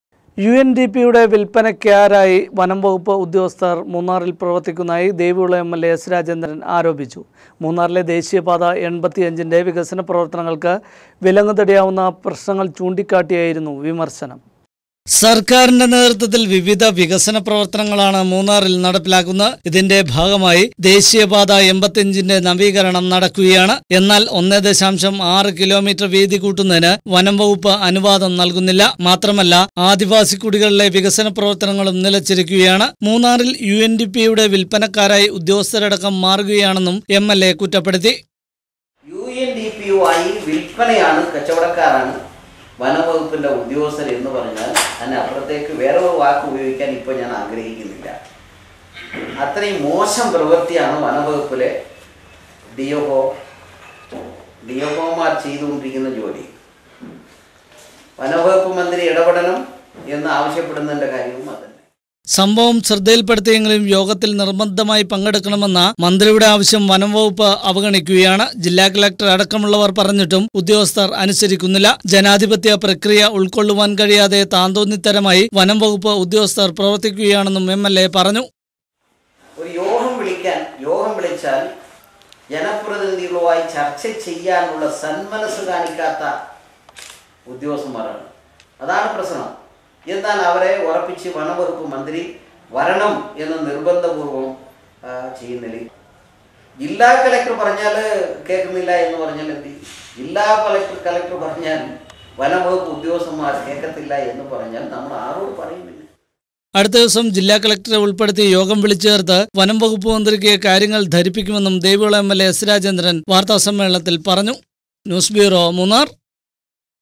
വനംവകുപ്പിനെതിരെ എസ്.രാജേന്ദ്രന്‍
യു.എന്‍.ഡി.പിയുടെ വില്‍പ്പനക്കാരായി വനംവകുപ്പ് ഉദ്യോഗസ്ഥര്‍ മൂന്നാറില്‍ പ്രവര്‍ത്തിക്കുന്നതായി ദേവികുളം എം എല്‍ എ എസ് രാജേന്ദ്രന്‍ പറഞ്ഞു.